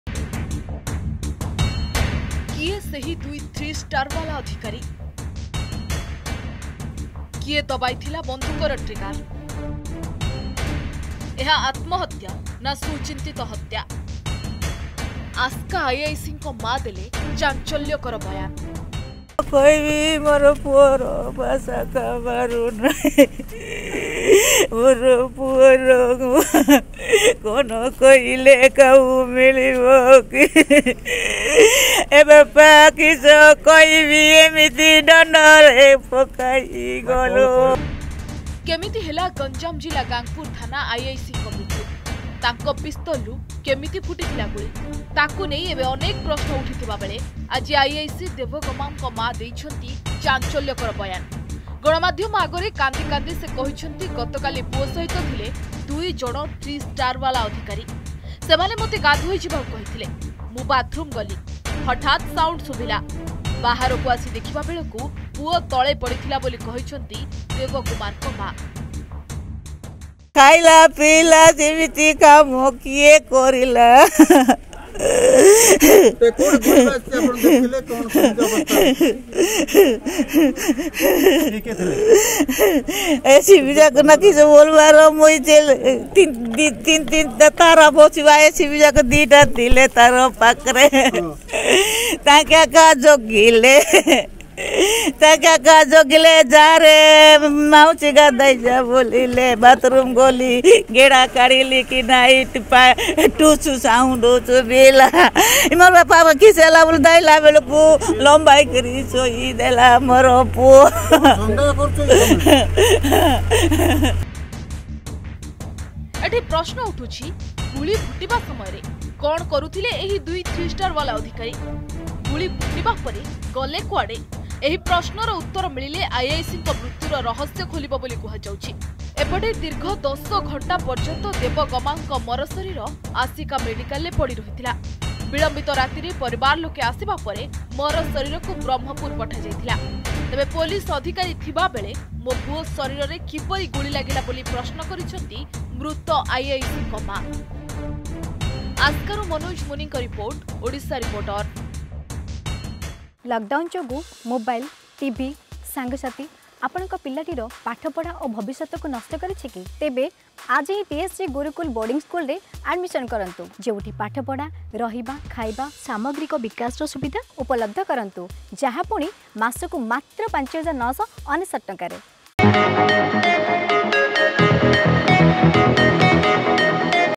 କିଏ ଦବାଇଥିଲା ବନ୍ଧୁକର ଟ୍ରିଗର ? || Gangapur Police IIC Deb Kumar Gamang Death News || Odisha Reporter
କେମିତି ହେଲା ଗଂଜାମ ଜିଲ୍ଲା ଗାଙ୍ଗପୁର ଥାନା ଆଇଆଇସିଙ୍କ ମୃତ୍ୟୁ ?  ତାଙ୍କ ପିସ୍ତଲରୁ କେମିତି  ଫୁଟିଲା ଗୁଳି ?  ତାକୁ ନେଇ ଅନେକ ପ୍ରଶ୍ନ ଉଠିଥିବା ବେଳେ ଆଜି ଆଇଆଇସି ଦେବ ଗମାଙ୍ଗଙ୍କ ମାଆ ଦେଇଛନ୍ତି ଚାଞ୍ଚଲ୍ୟକର ବୟାନ । ଗଣମାଧ୍ୟମ ଆଗରେ କାନ୍ଦି କାନ୍ଦି ସେ କହିଛନ୍ତି, ଗତକାଲି ପୁଅ ସହିତ ଥିଲେ ଦୁଇ ଜଣ ଥ୍ରୀ ଷ୍ଟାର ବାଲା ଅଧିକାରୀ ।